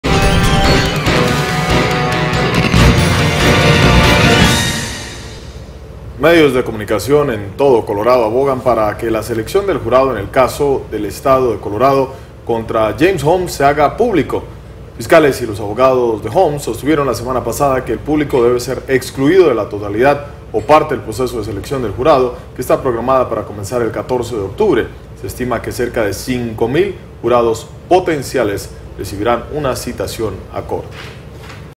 Caso James Holmes
Denver, (Entravision).- Medios de comunicación de Colorado abogan para que la selección del jurado en el caso del estado de Colorado contra James Holmes se haga pública.